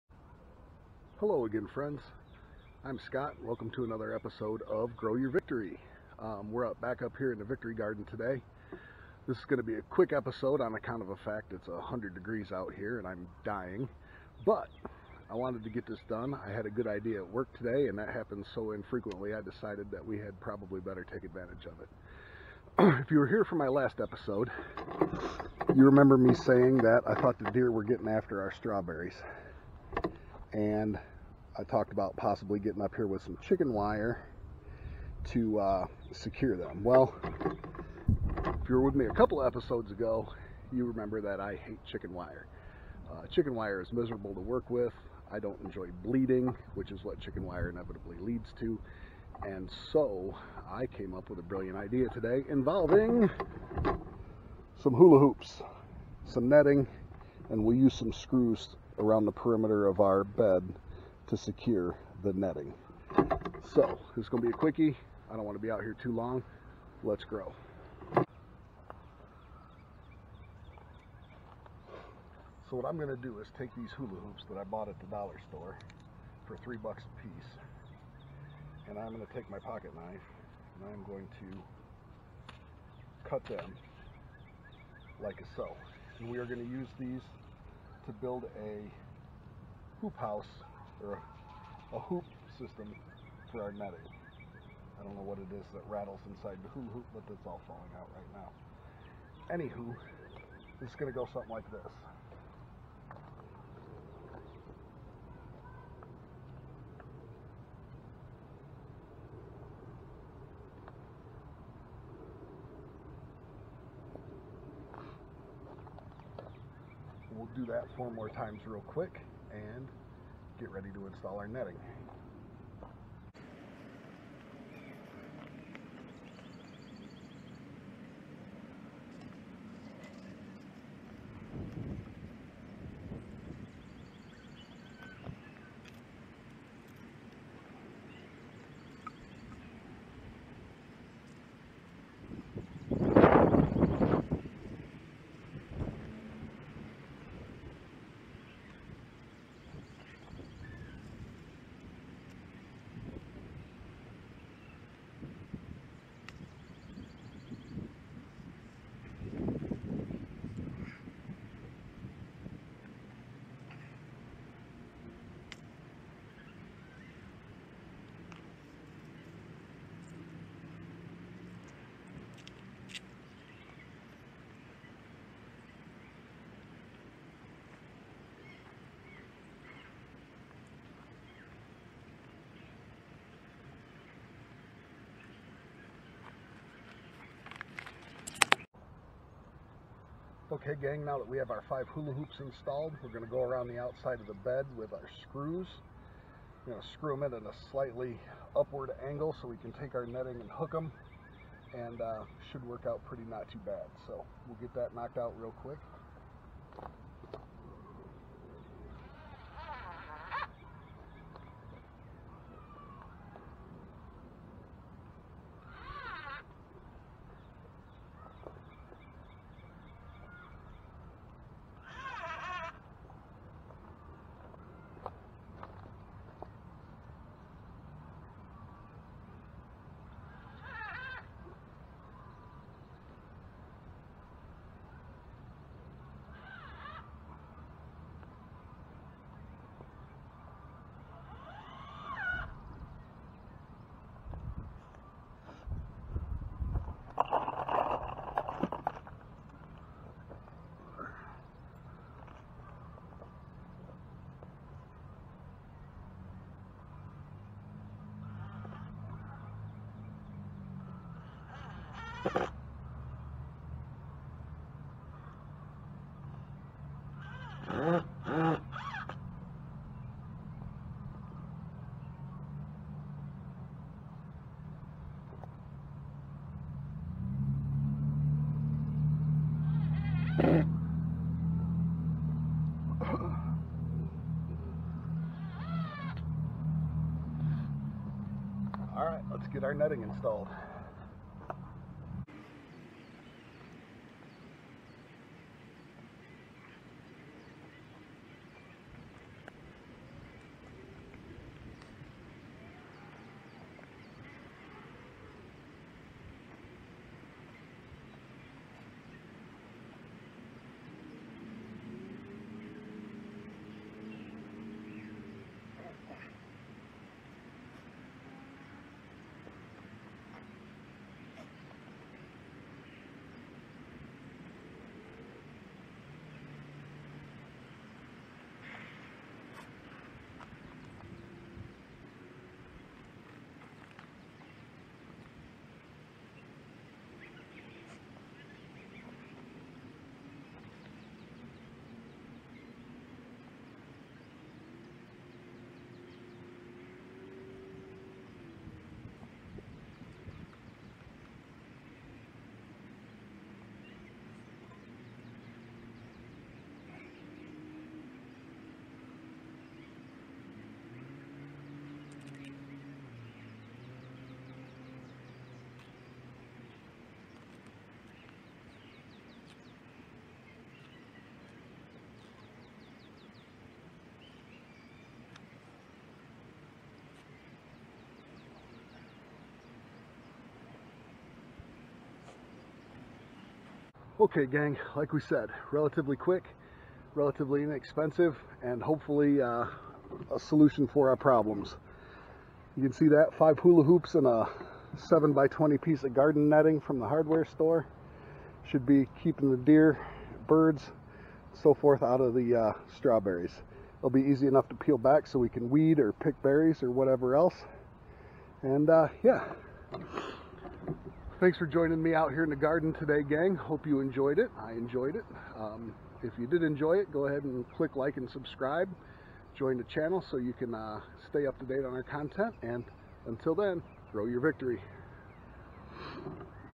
Chicken wire alternative with garden netting and Hula Hoops. A quick project on a 100 degree day!
In this episode I came up with a great alternative to using chicken wire to keep the deer and birds out of our strawberry bed.  I used some Hula Hoops and garden netting to create a hoop frame to protect our berries!